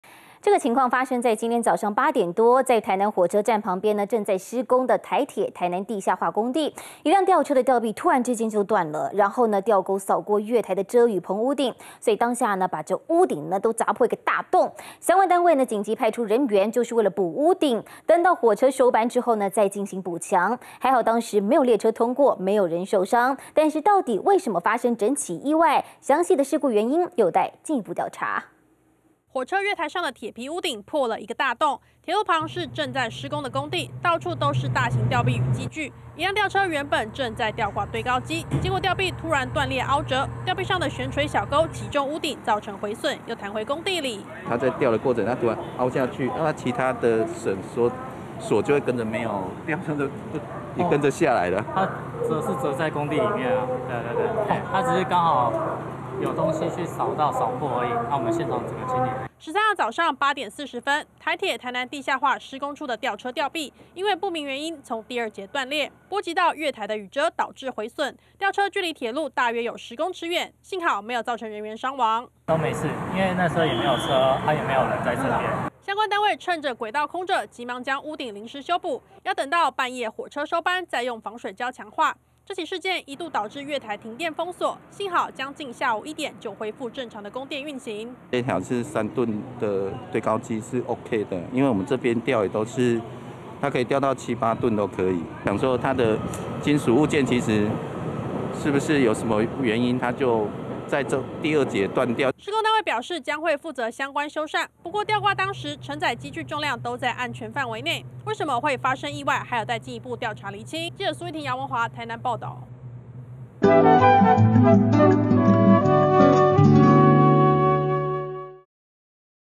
離鐵路僅十公尺！ 工地吊臂斷裂「砸破」月台屋頂
今（13）日上午8時許，台南火車站旁，正在施工的台鐵台南地下化工地。一輛吊車的吊臂突然斷裂，吊鉤掃過月台遮雨棚屋頂，將屋頂砸出一個大洞。對此相關單位緊急派出人員進行修補，等到火車收班後，將再進行補強。幸好當時沒有列車通過，也沒有造成人員傷亡，但意外發生原因，還有待進一步調查釐清。